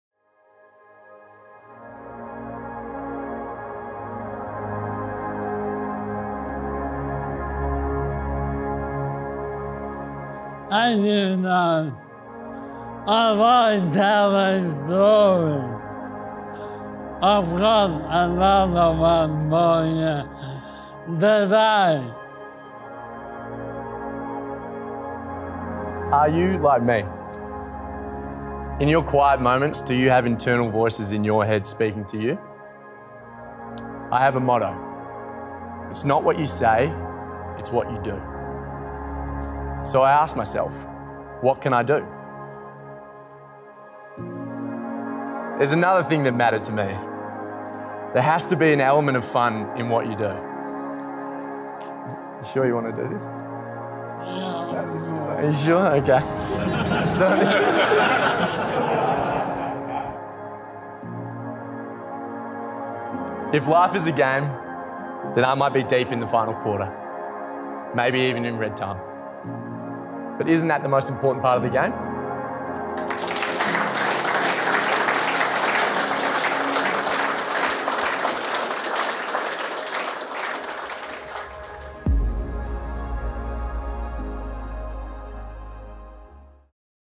Neale Daniher - FightMND 2021 Play On speech teaser
Every year Neale gives an inspirational and entertaining speech to Melbourne and Collingwood AFL teams. In 2021, with son Ben, Neale also shared his wisdom with West Coast and Essendon.

This is an inside glimpse behind the story of 'Play On''.